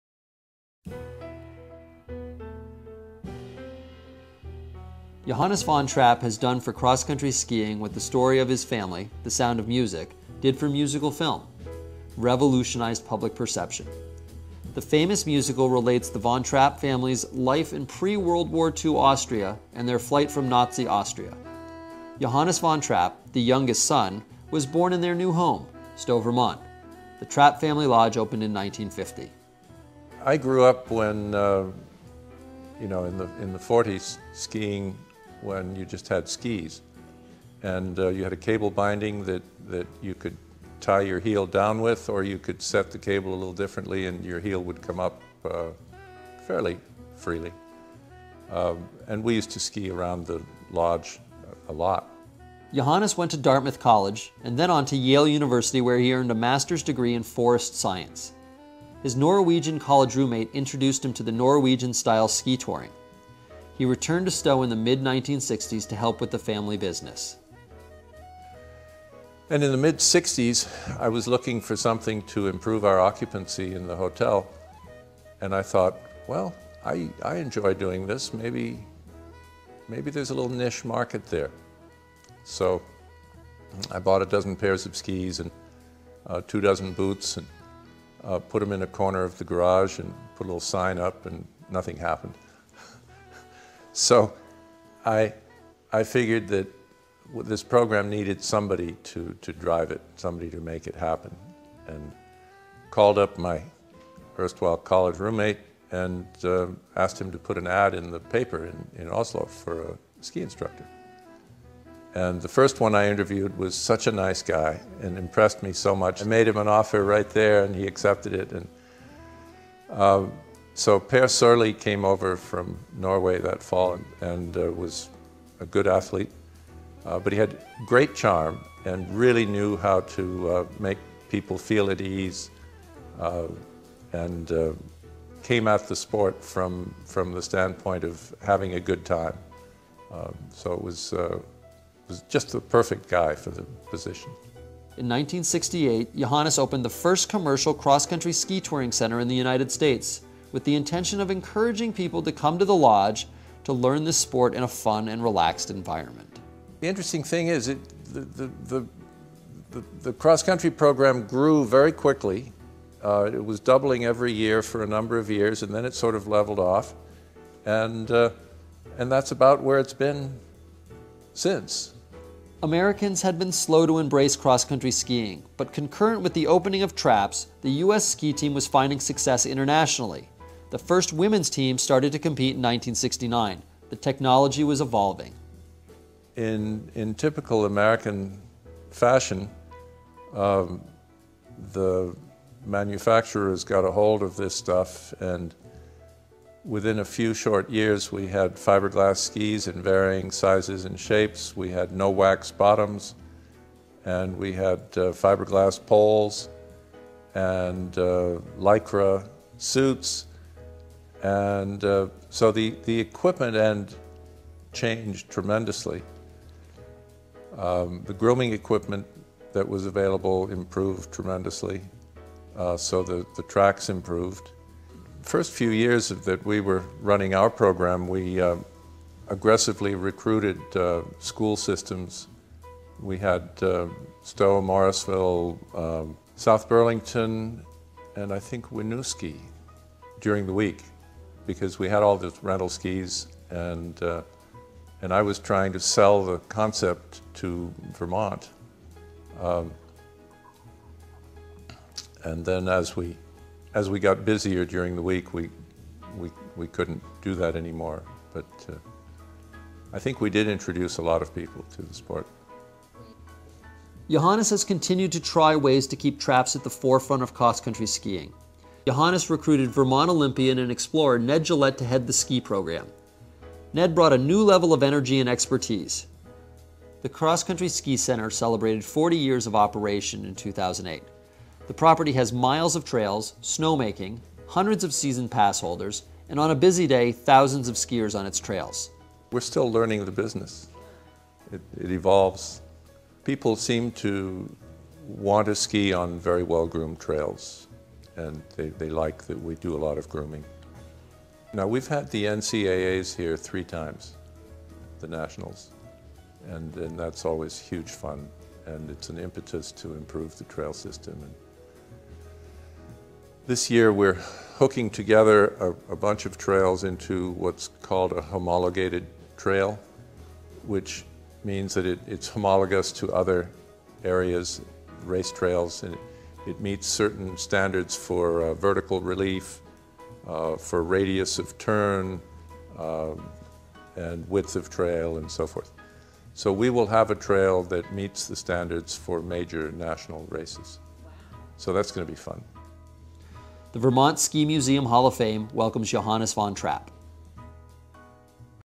Johannes von Trapp - 2010 Inductee
The story of Johannes von Trapp’s family has been well-documented, but his contributions to Nordic skiing’s development should not go unrecognized. When, in the mid 60s, he was looking for ways to improve the Lodge’s occupancy, he thought he could develop a niche market by purchasing some cross country skis and bringing a Norwegian ski instructor to Trapps to drive the program. This was the beginning of the first commercial cross country ski center in the United States.